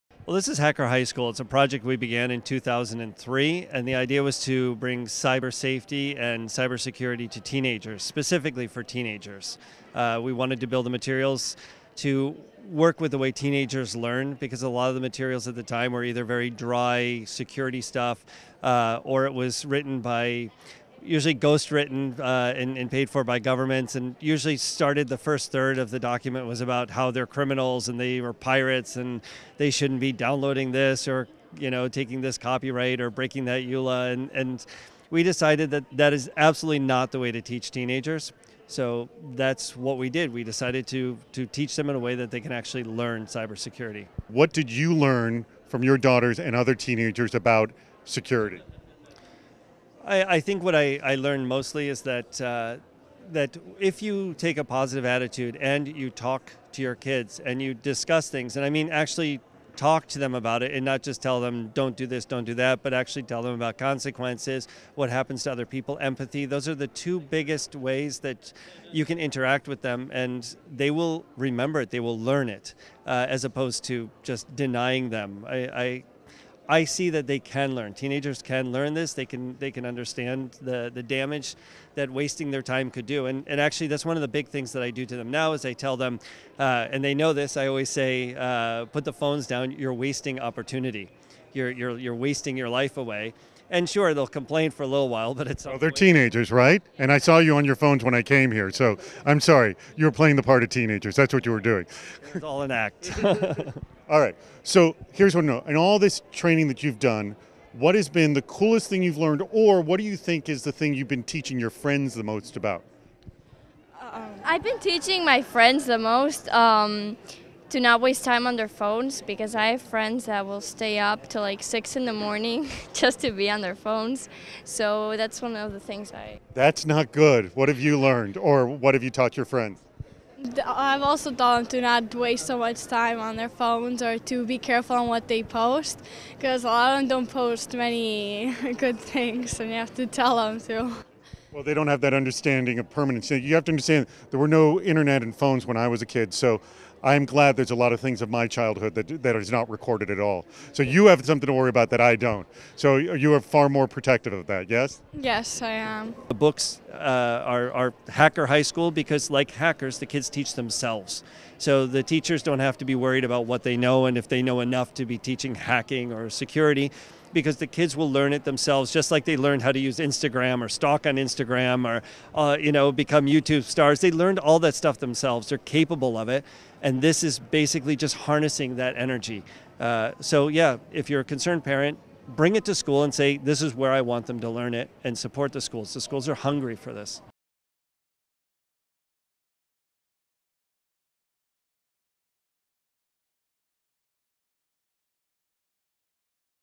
Hacker High School: Our Next Generation of Security Professionals
Teenagers don’t learn cybersecurity by being scared. They learn by getting their hands dirty and hacking.

“You’re a criminal” was often the accusation of much of the government-sponsored teenage-based security training. In the early 2000s, the days of Napster, most people just assumed teenagers were illegally downloading music. The only way to thwart that behavior was to chronically scold teenagers for their behavior.

But that’s not the way teenagers learn, realized Pete Herzog, Managing Director of ISECOM, who helped initiate a cybersecurity and safety training effort called Hacker High School back in 2003.